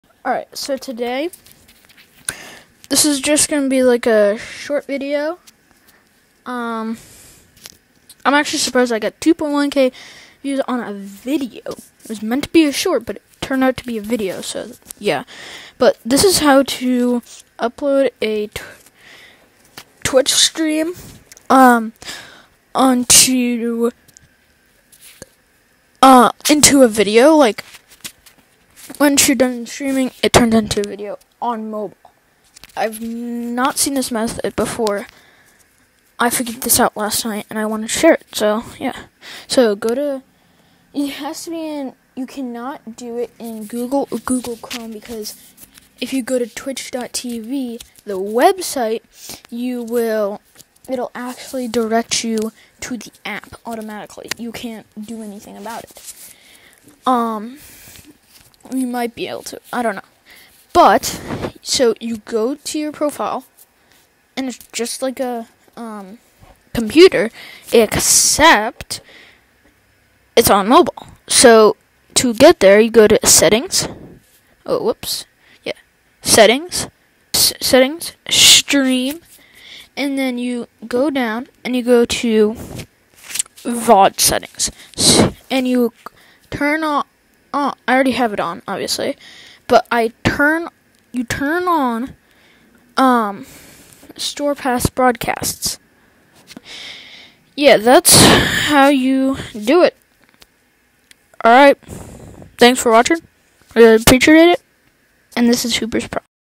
How to Upload Videos on Twitch with a Mobile Device
In this short video I explain how to upload a video to twitch, after you stream. This is a one of a kind new method that is guaranteed to work on any iOS device and most likely on android too!